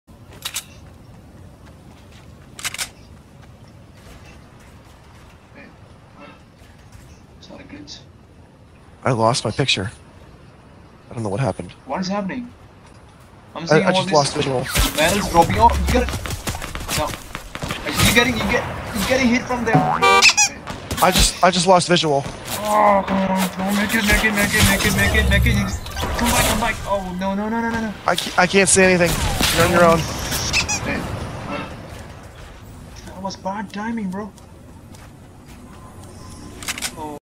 When the Stormtroopers mask goes dark
One day, while adventuring with default @Addix, my mask went completely dark. To my horror... What witchcraft is this?!!!